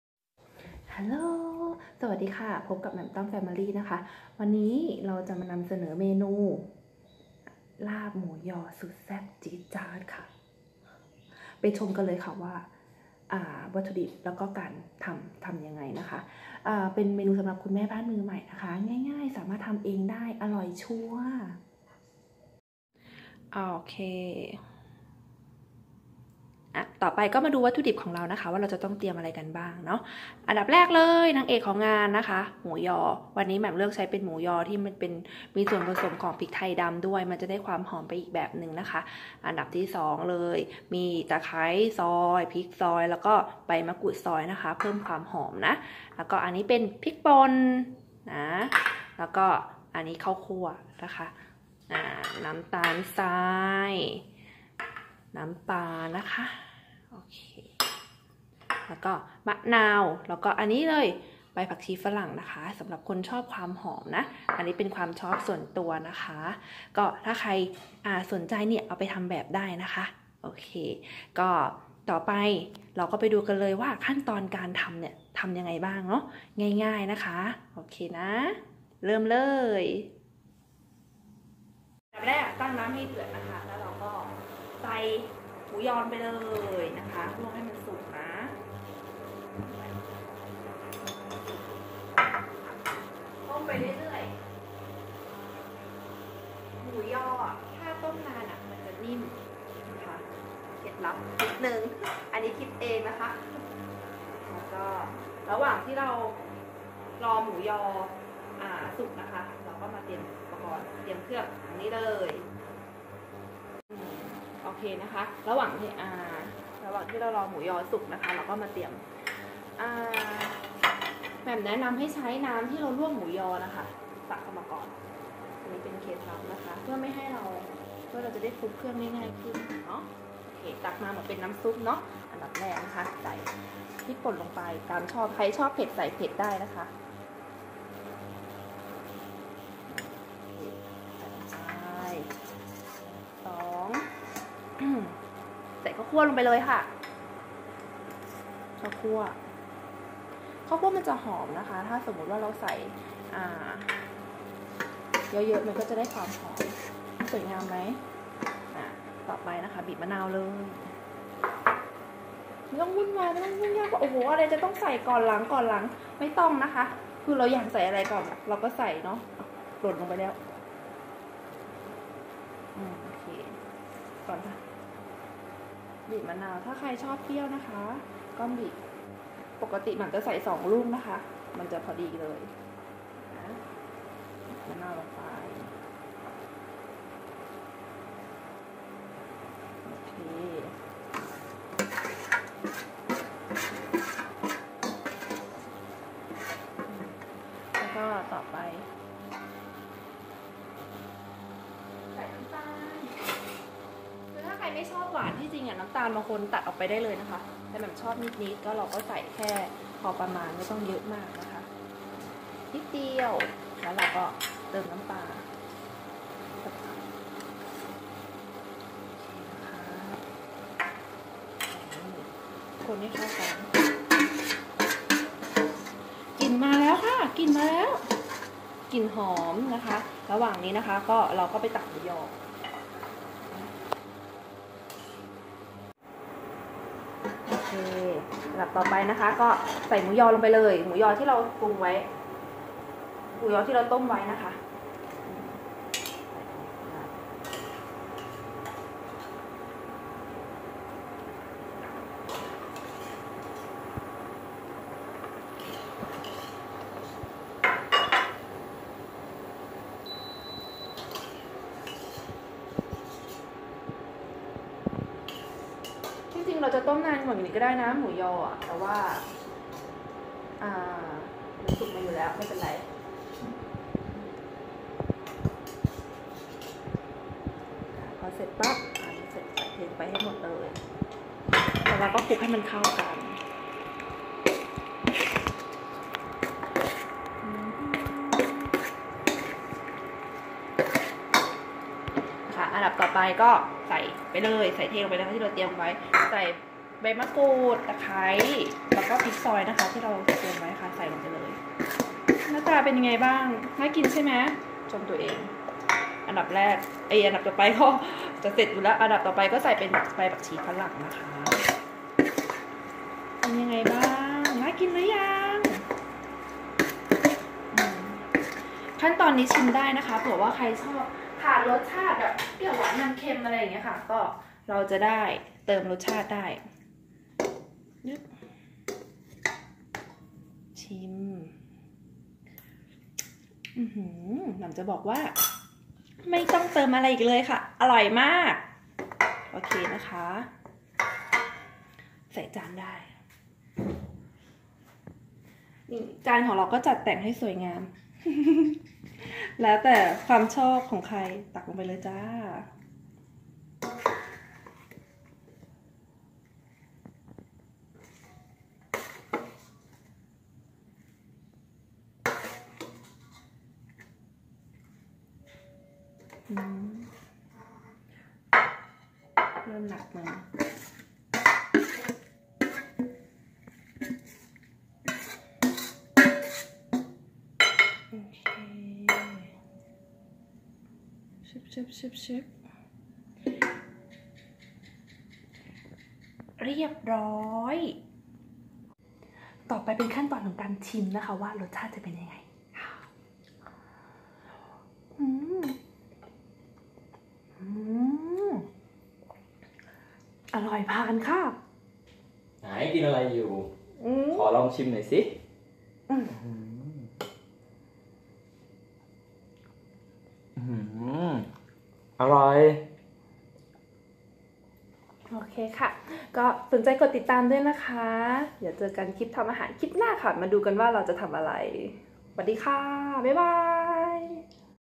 ลาบหมูยอแซ่บจี๊ด ทำง่ายๆที่บ้านกินกับข้าวก็อร่อยหรือเป็นกับแกล้มก็เข้ากันดี๊ดี
คลิปนี้เราจะมาทำ ลาบหมูยอแซ่บจี๊ด สำหรับแม่บ้านมือใหม่หัดเข้าครัวจ้าาาา
มาดูวัตถุดิบเลย!
- หมูยอ กะปริมาณทานได้ 2 คนนะคะ หรือจะมากกว่านี้ก็ได้ค่ะ
- เครื่องปรุง คือ พริกป่น ข้าวคั่วป่น น้ำตาล น้ำปลา (ยี่ห้อตามใจชอบเลย)
- ตะไคร้ซอย ใบมะกรูดซอย พริกสดซอย
- ใบผักชีฝรั่ง หั่นเป็นชิ้นๆ พอสวยงาม
- ผักสดเครื่องเคียงตามความชอบ